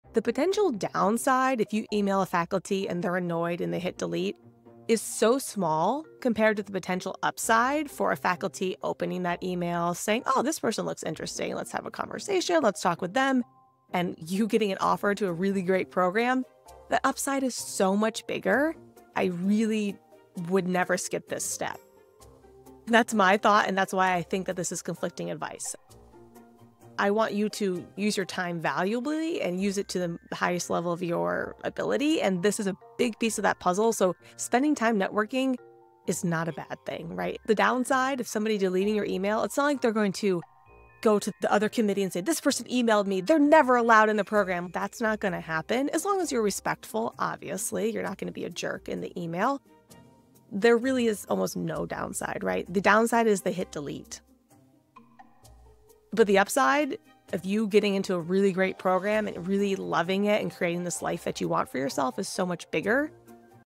does networking help or hurt your PhD chances?
is networking worth it?

Or is it annoying and you should skip it?